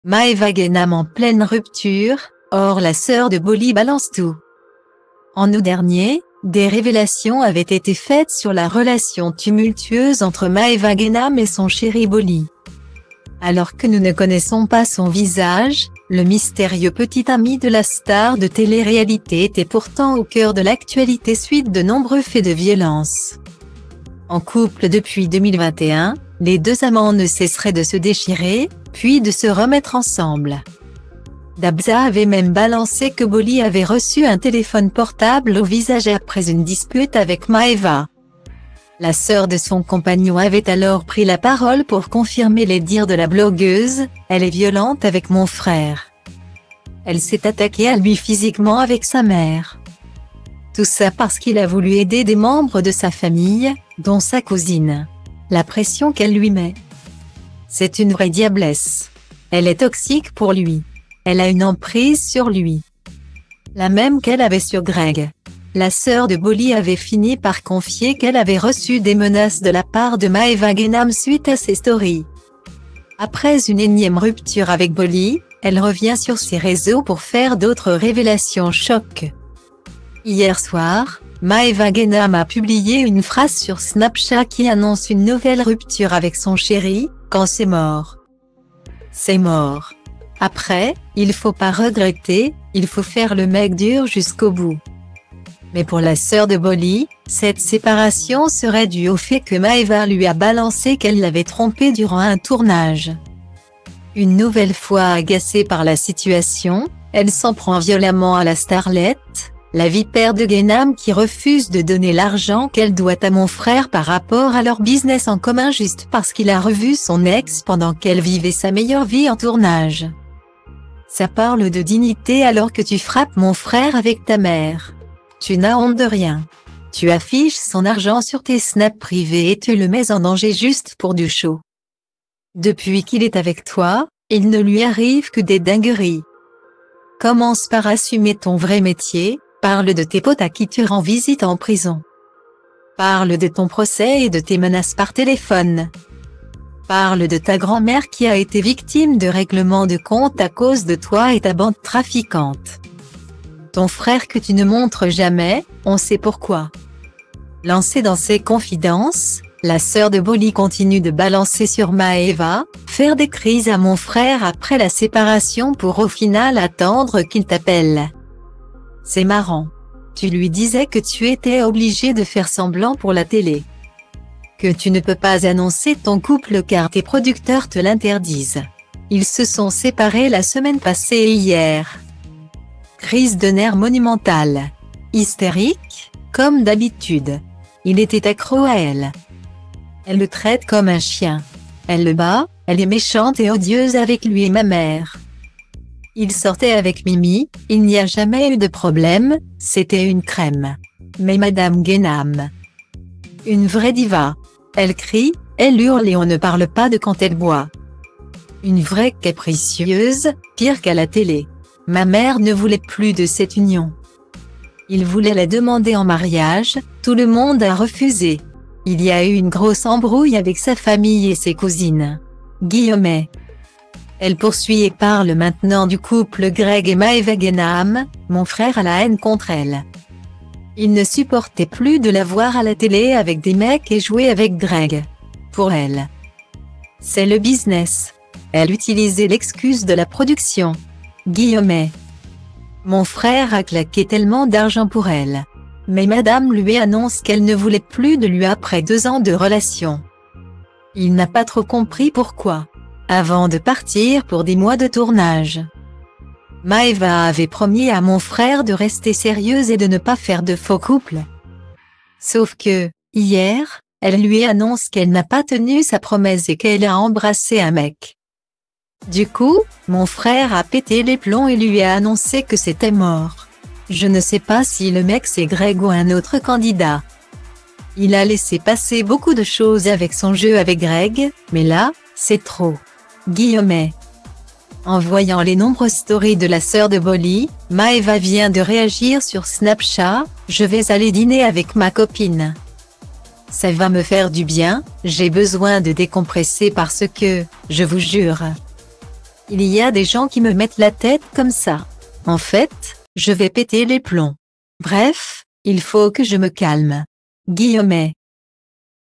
Maeva Ghennam en pleine rupture : hors la sœur de Boli balance tout
En août dernier, des révélations avaient été faites sur la relation tumultueuse entre Maeva Ghennam et son chéri Boli.Alors que nous ne connaissons pas son visage, le mystérieux petit ami de la star de télé-réalité était pourtant au cœur de l'actualité suite de nombreux faits de violence.En couple depuis 2021, les deux amants ne cesseraient de se déchirer, puis de se remettre ensemble.